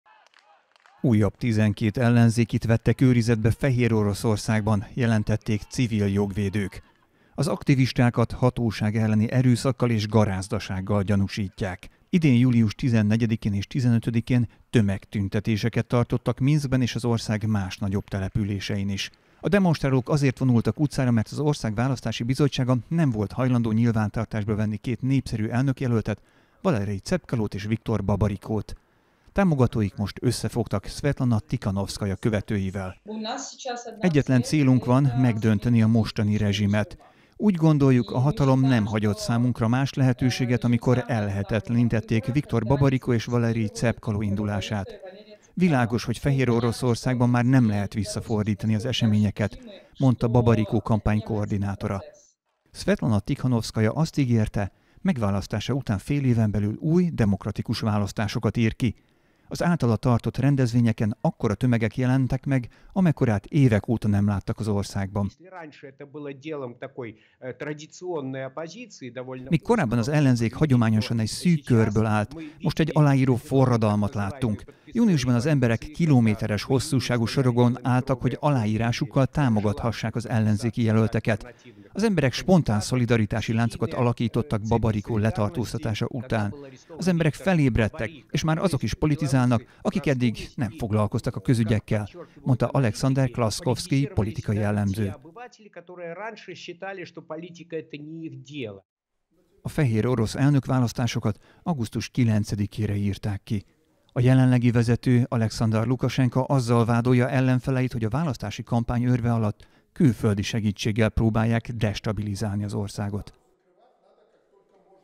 Egységbe forr a fehérorosz ellenzék
Egységesen lép fel a fehérorosz ellenzék, miután több jelöltjüket is eltiltották az elnökválasztáson való indulástól.